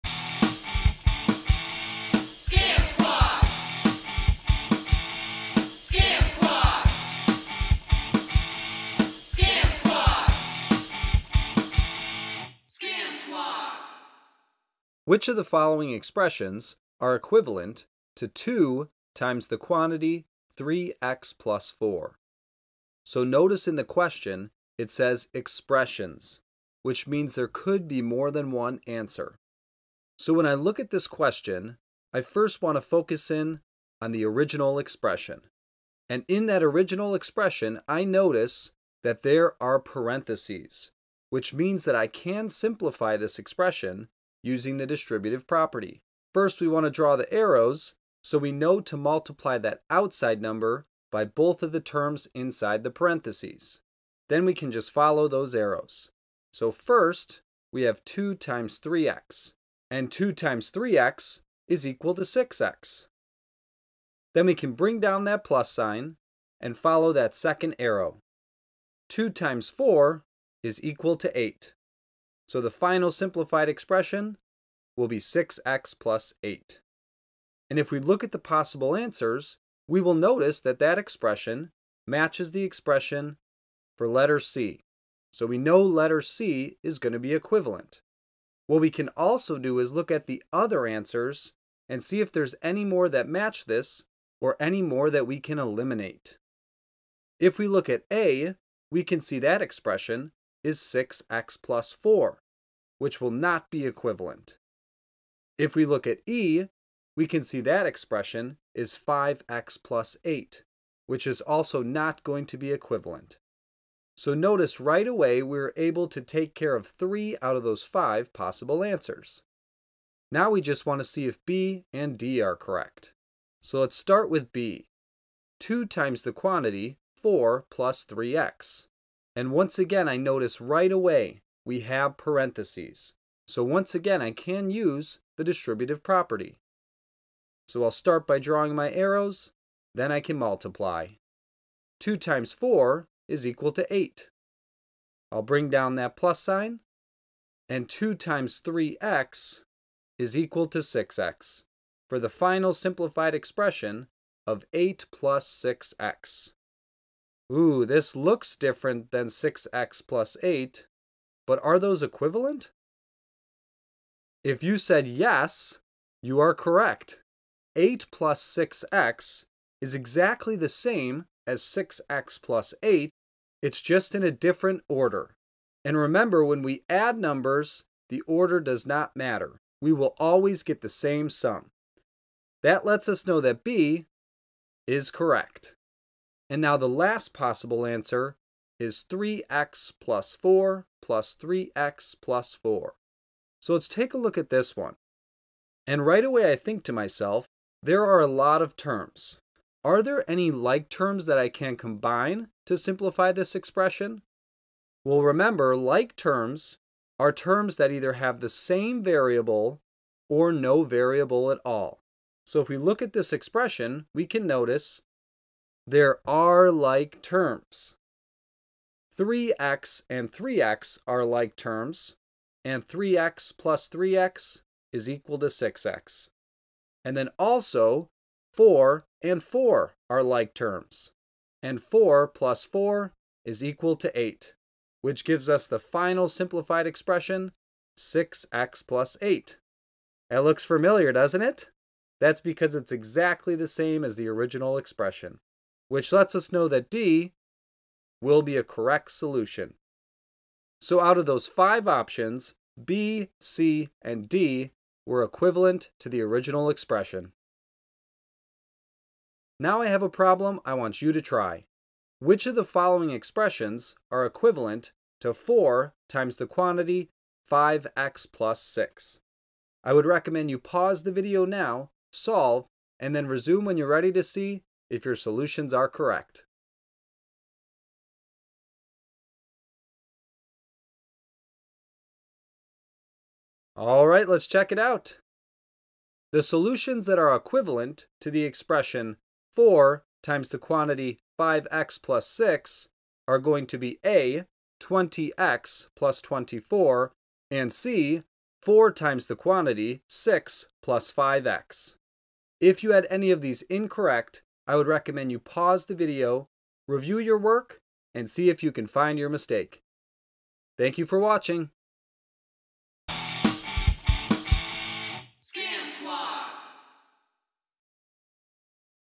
How to Identify Equivalent Expressions | Combining Like Terms | Distributive Property
This video shows viewers how to identify equivalent expressions using the Distributive Property and Combining Like Terms.

I would recommend this video for students in 6th grade math or 7th grade math.